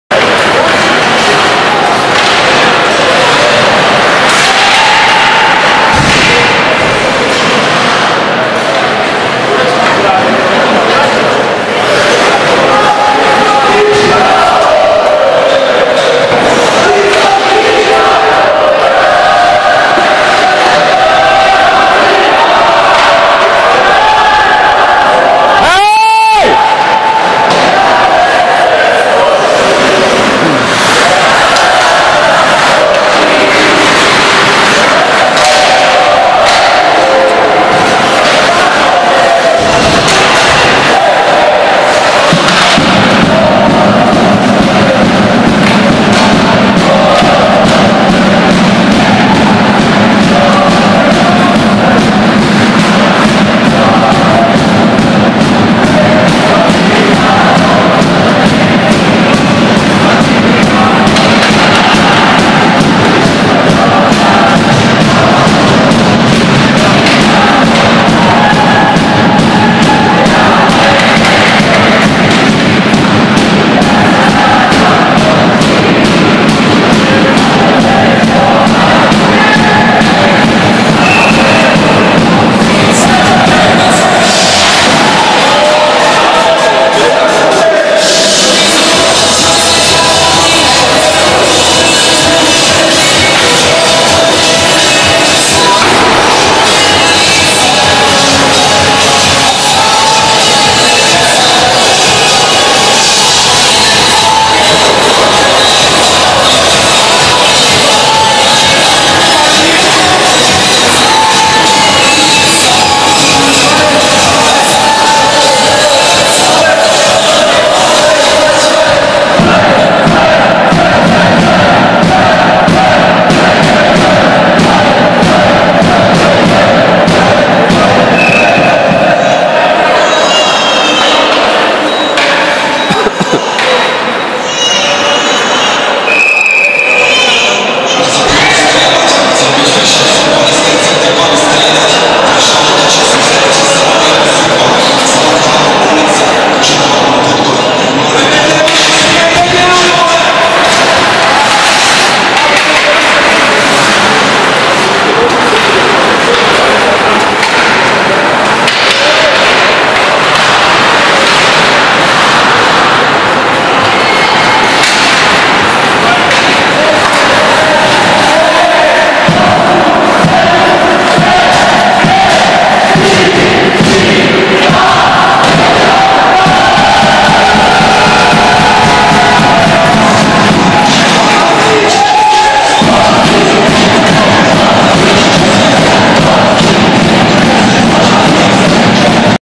Finálový zápas HC Francova Lhota : HC Sharks
Finálový zápas HC Francova Lhota : HC Sharks, jasná výhra, oslava 1. místa ve skupině D a autobusová účast fanoušků :-).